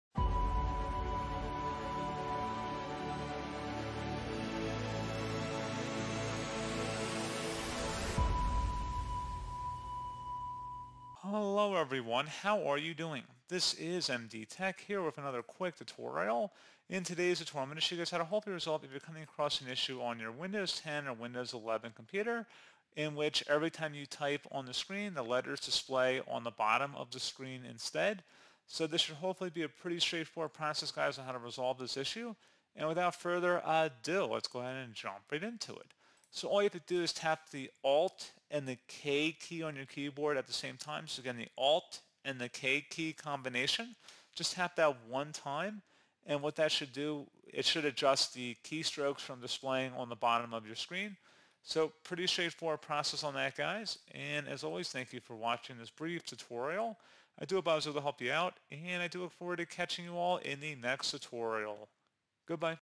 Every Time I Type the Letters Display at the Bottom of the Screen in Windows 11 [Guide]
It is not uncommon for the keyboard to malfunction from time to time. And while most of the time a key will go dead, sometimes the keys begin to register on their own as if the keyboard has gone crazy.

Such an issue stems from both the hardware and operating system sides. If you are experiencing a similar issue, we'll cover some fixes you can try to get it back on track.

Issues addressed in this tutorial:
letters showing up on screen
letters on screen
displays at the bottom of the desktop

It is really annoying when the keyboard is typing wrong letters/characters/symbols. How do I change my keyboard keys back to normal in Windows 10/11?